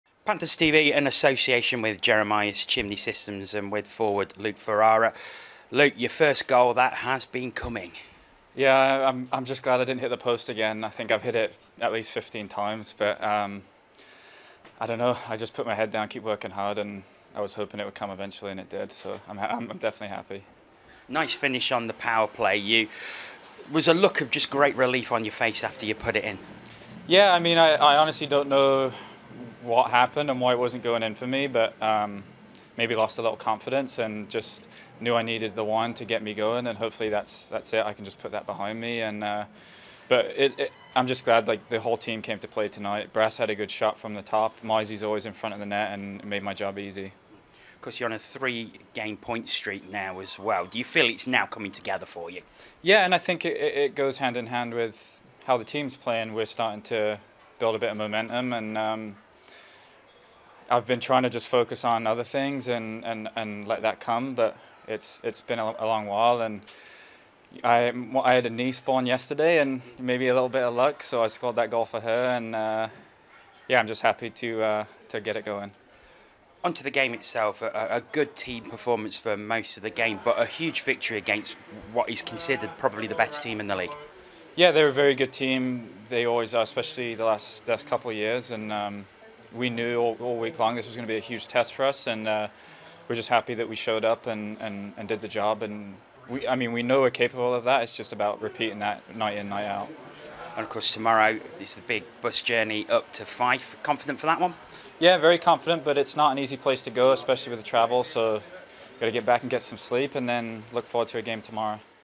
Ferrara: That goal was for my baby niece
Forward Luke Ferrara spoke with Panthers TV - in association with Jeremias Chimney Systems - as The Nottingham Panthers beat Belfast Giants 5-4 in the Elite League at the Motorpoint Arena on Saturday night - and dedicated his first goal for the club to his baby niece born yesterday.